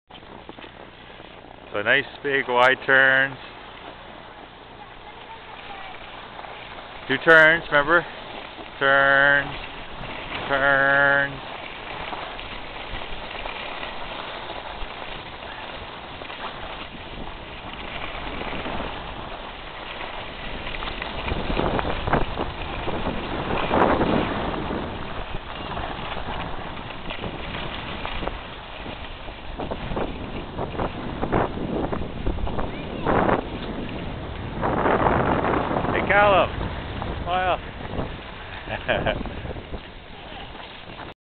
C Skiing at Snow Valley
This is an action video of C skiing for the first time at Snow Valley.  He did the snow plow right down the middle of the hill.  No turns.  We had a great day.